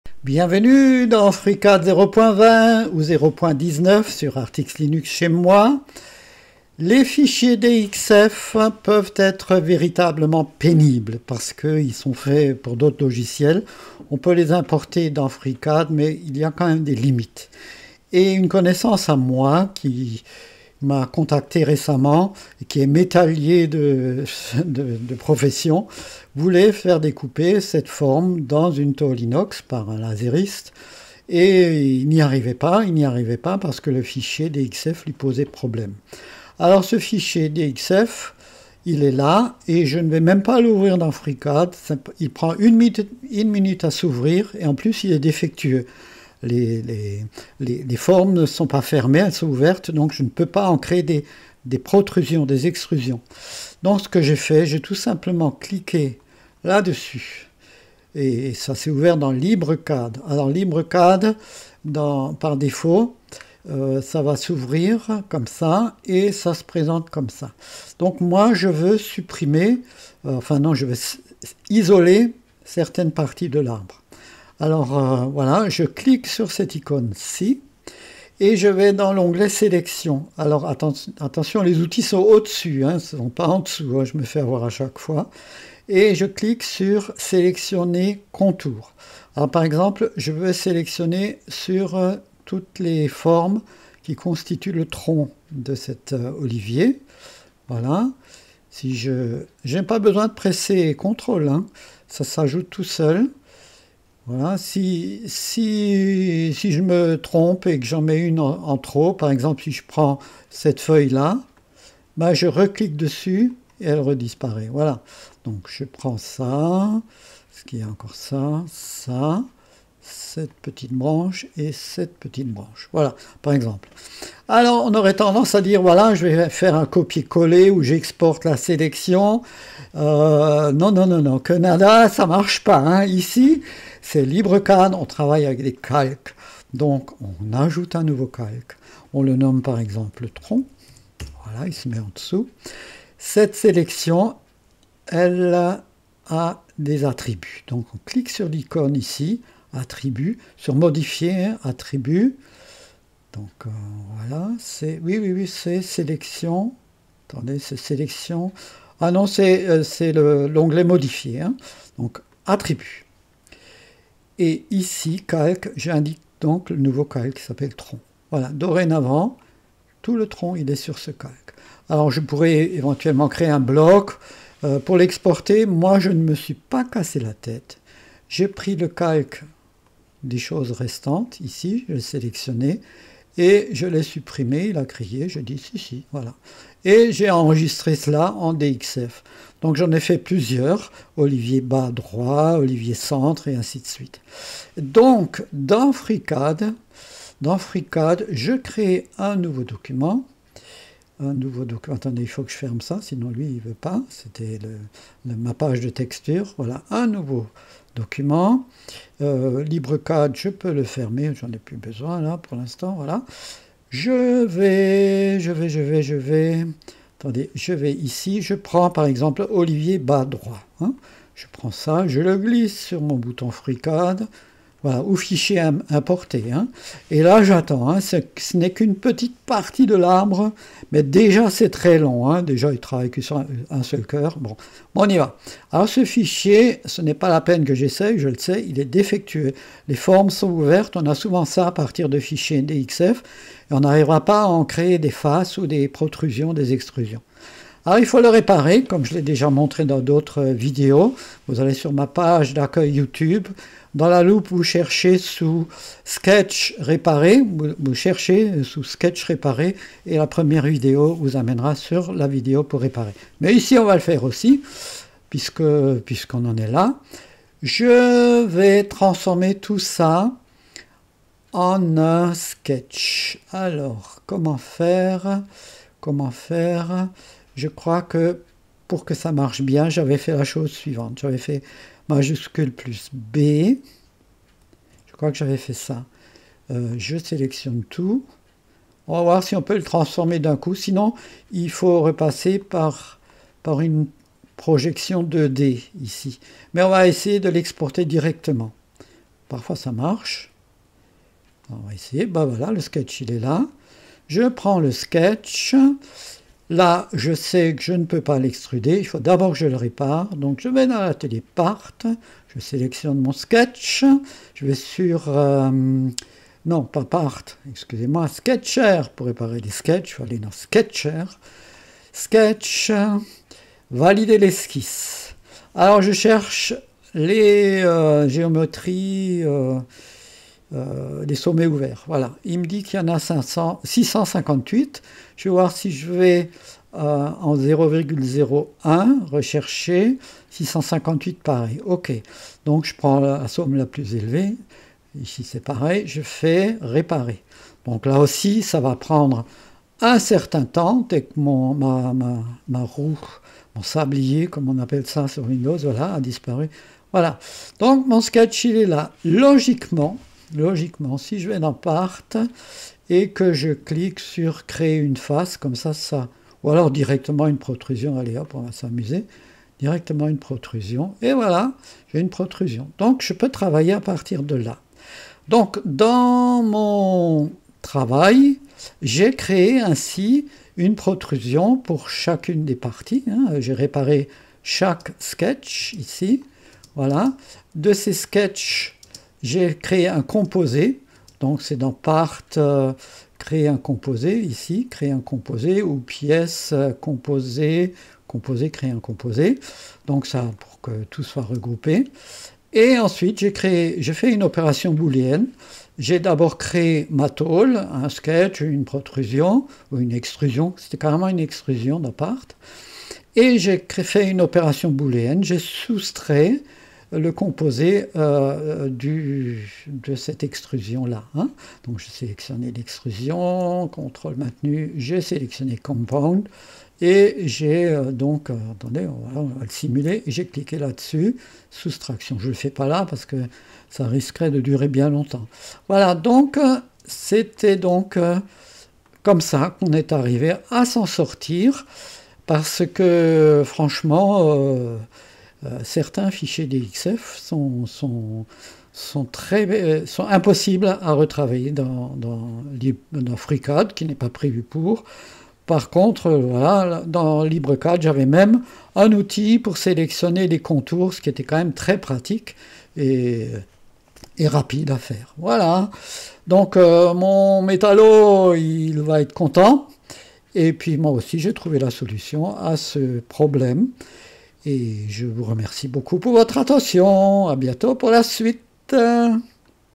FreeCAD - LibreCAD : Importer, diviser et réparer un "gros" fichier DXF
Note : Le but de la procédure est de créer une vue 3D pour la présenter au client.
Pour la découpe laser seule, il suffit dans LibreCAD de tracer un rectangle autour de l'arbre et d'envoyer le fichier DXF au "laseriste".

FreeCAD 0.20 et LibreCAD 2.1.3 :
- Sélectionner une multitude de détails dans LibreCAD pour diviser un
- "gros" fichier en plusieurs petits fichiers.
- "Réparer" ces petits fichiers dans FreeCAD.
- Réassembler ces petits fichiers réparés dans FreeCAD.
- Préparer la forme pour la découpe au laser ou l'impression 3D
- Exporter au format DXF